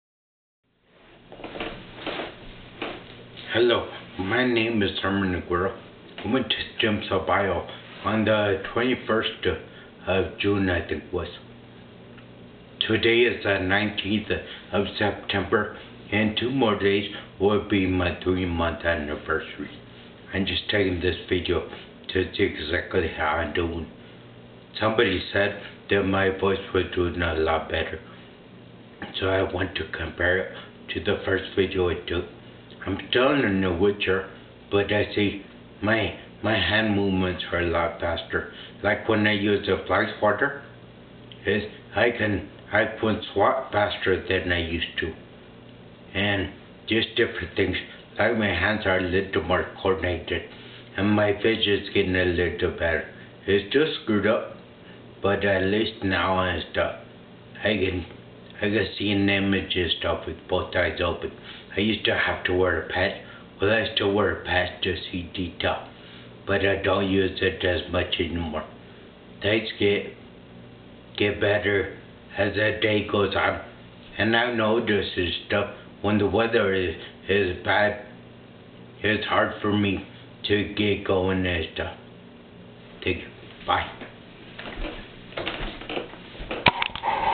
Stem Cell Biotherapy 3 mo. after treatment
This video was made 3 months after my trip to the west coast.  I had just entered the point I could start improving.  When I got the procedure done I was told it would be 3-6 mo. Before I started improving.